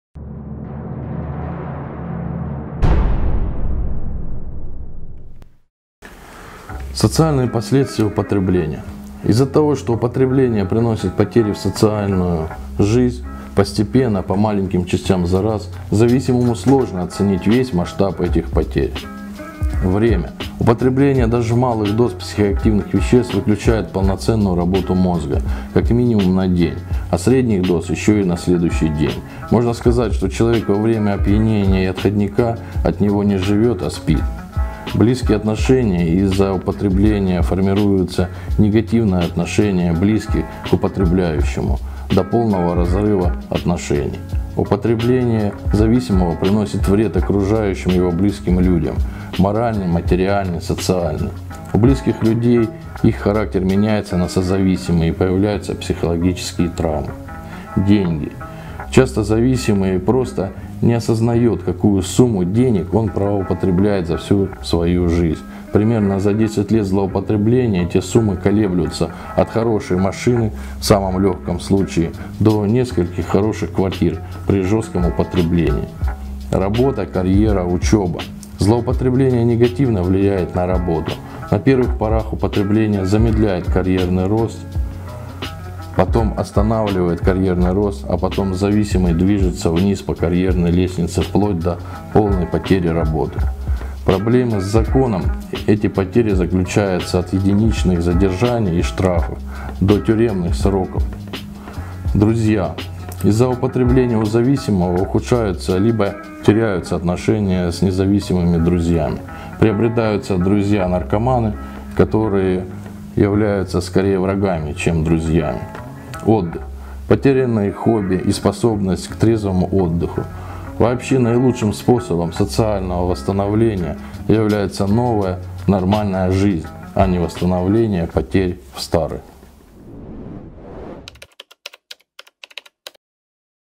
Социальные последствия употребления
Магистр педагогического образования, психолог, реабилитолог, руководитель РООО "Ростов без наркотиков" С.В. Горяинов о социальных последствиях зависимого человека.

Из-за того, что употребление приносит потери в социальную жизнь не сразу, а постепенно, зависимому человеку сложно оценить весь масштаб этих потерь.